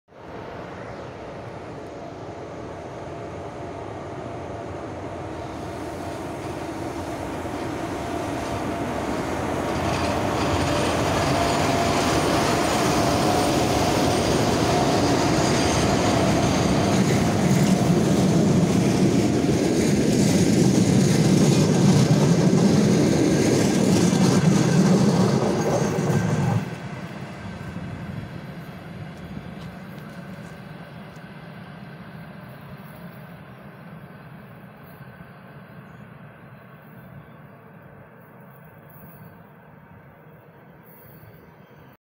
"песок из Чс4-107"карантин=Развозка жд из-за отмены пригородных 7608 Тетерев-Киев-Дарница пл.Ворзель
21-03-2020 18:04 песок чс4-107
"Карантинная Развозка" сменных жд из за отмены пригородных 7608 Тетерев-Киев-Дарница пл. Ворзель перегон Немешаево-Буча.